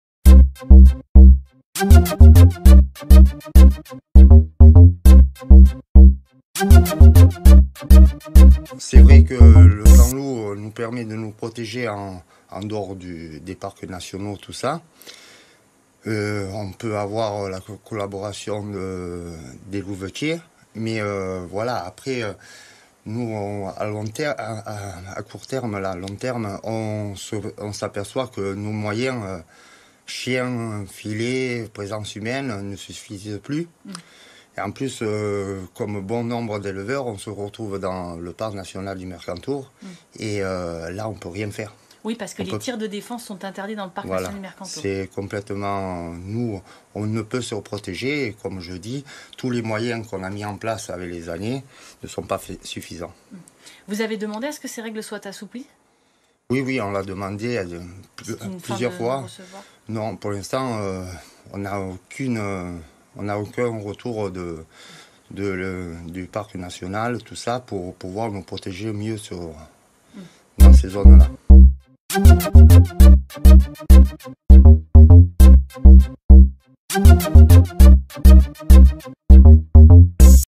Des moyens contre la prédation du loup qui ne suffisent plus
Mickaël Viale, berger dans la Roya et membre de la Fédération départementale ovine, est l'invité de Bonjour la Côte d'Azur jeudi 26 octobre 2023.